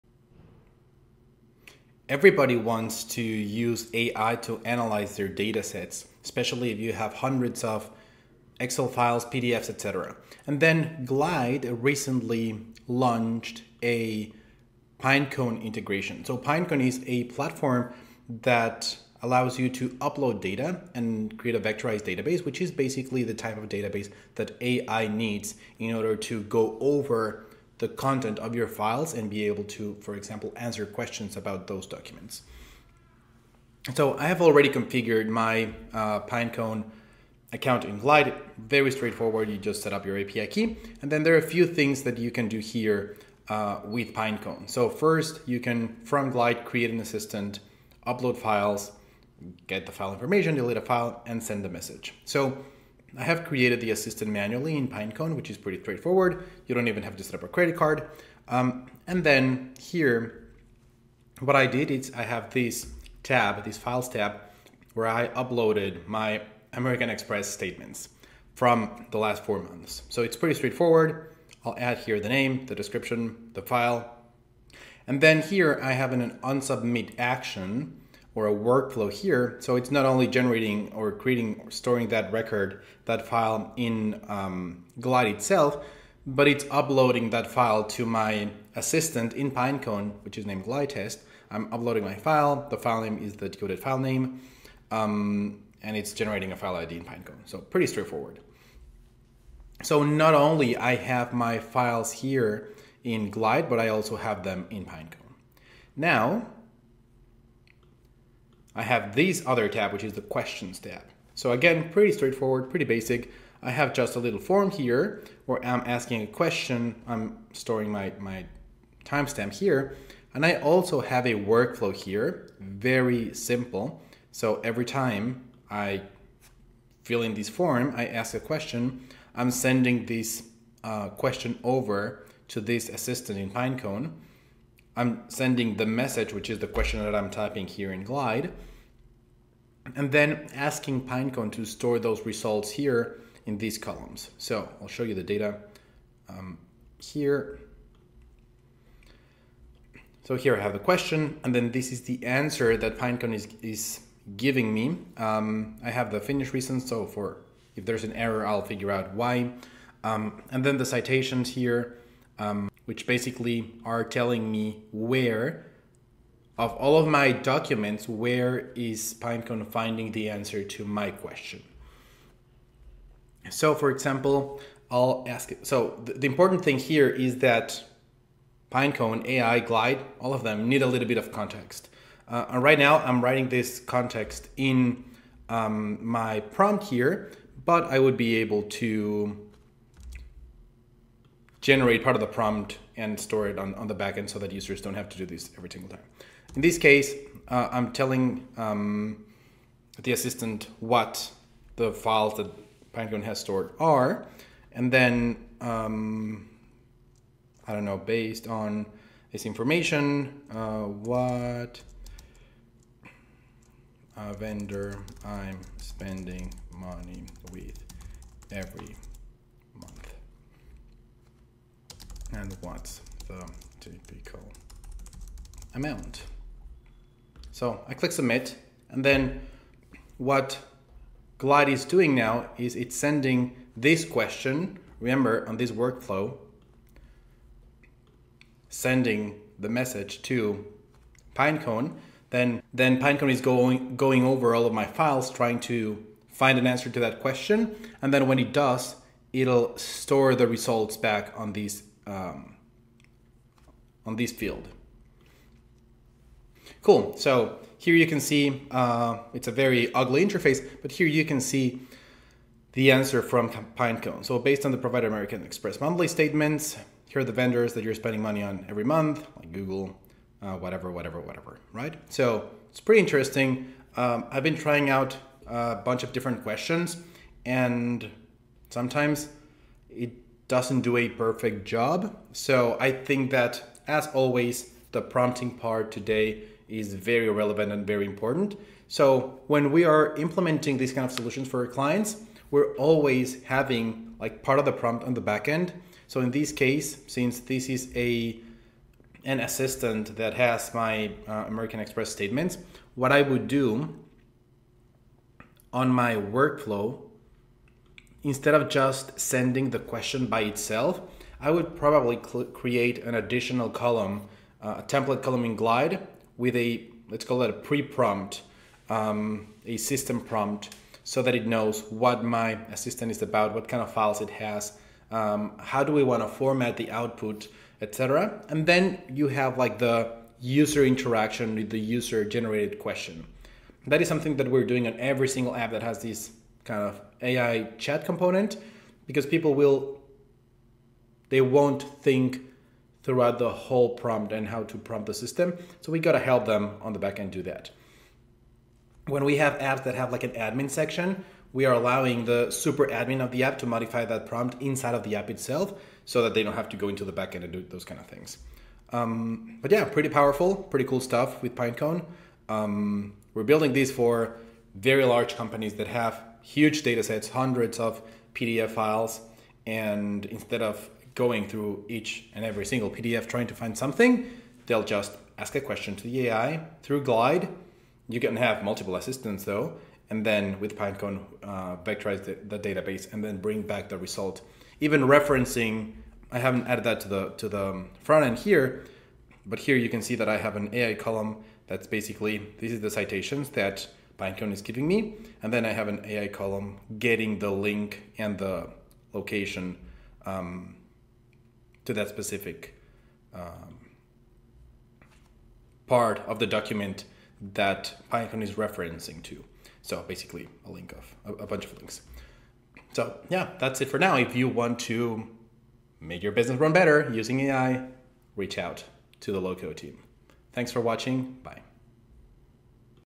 Build a No-Code AI Agent with Glide + Pinecone | Search PDFs & Business Docs Instantly
Tired of spending hours searching through hundreds of business documents? With Glide and Pinecone, you can build a no-code AI agent that instantly searches PDFs, Excel files, and other business docs—giving you answers in seconds.

In this tutorial, you’ll learn how to:

● Set up a vectorized database using Pinecone (no credit card required)
● Build AI assistants in Glide to analyze business data
● Upload and manage multiple files (PDF, Excel, Word) seamlessly
● Use advanced prompting strategies for accurate AI responses
● Add admin controls for team usage
● Create citations so you know exactly where answers come from

📌 Who this is for:

● Business owners and managers who want faster document insights
● Teams struggling with scattered knowledge and slow research
● Companies needing AI-powered business intelligence

Real-world demo included: I show how this system analyzes my own American Express statements—just like enterprises are already doing with thousands of PDFs.